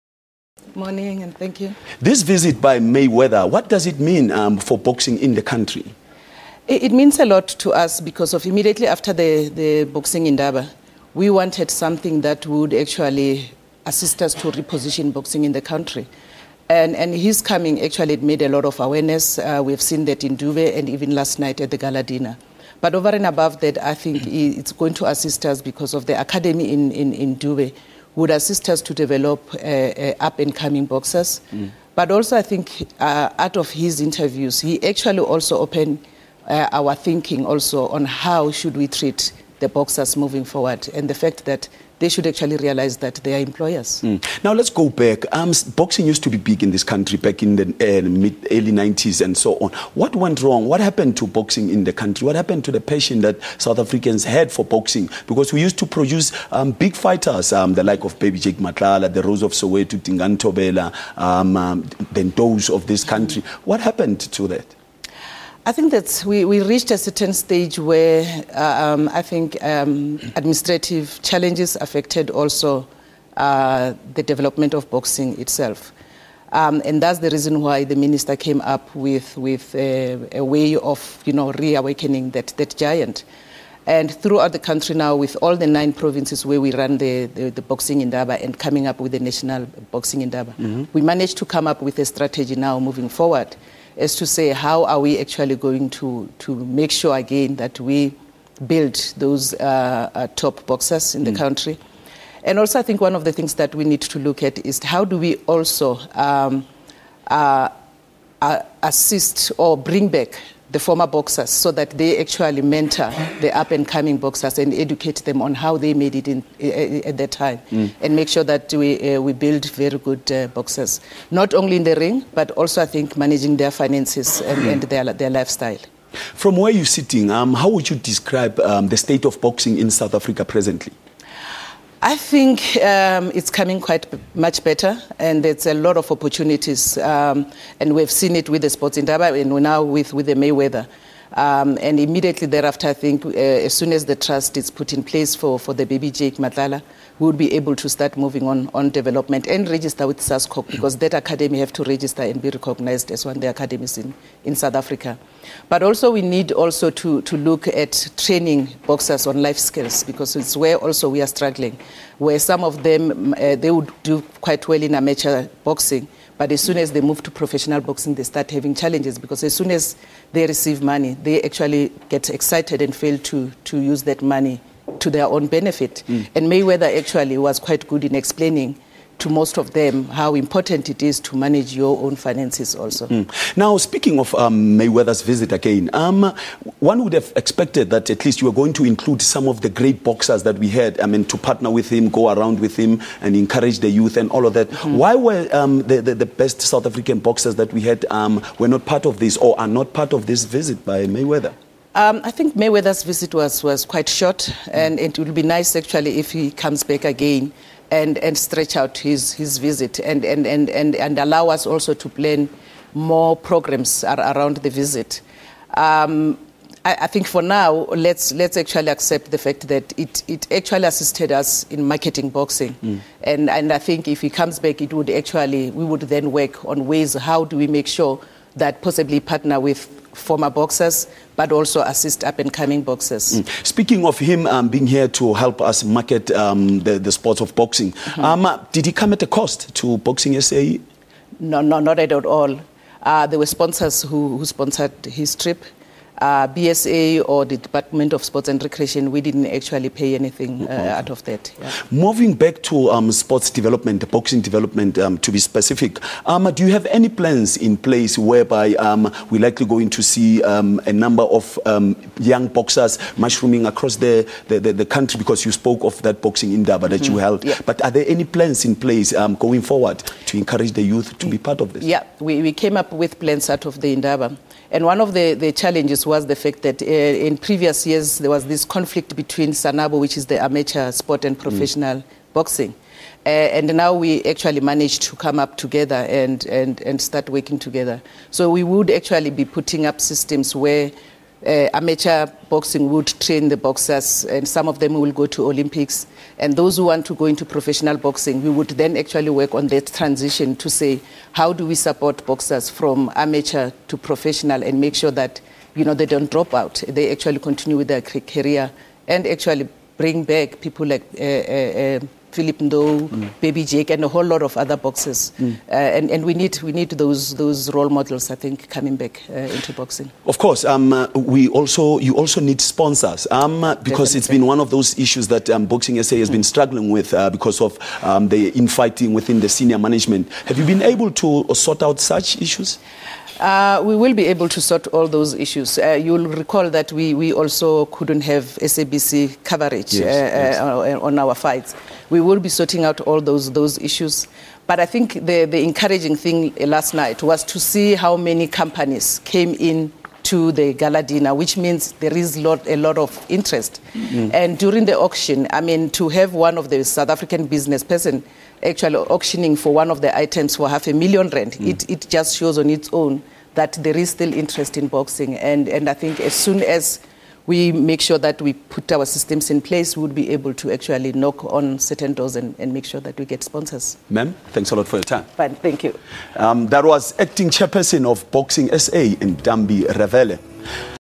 Ravele on Mayweather's visit and other boxing issues
The spirit of a sleeping boxing giant has been reawakened with world champion Floyd Mayweather Junior's visit to Soweto. Mayweather's visit was in part to honour legendary South African boxer, the late Baby Jake Matlala. Matlala died last month. Mayweather, together with Sports Minister Fikile Mbalula and Matlala's family, unveiled a plaque in his honour at the Dube Boxing Community Hall. The facility will be renamed after Matlala. To talk to us about reawakening boxing in the country we are joined in the studio by the Acting Chairperson of Boxing SA, Ntambi Ravele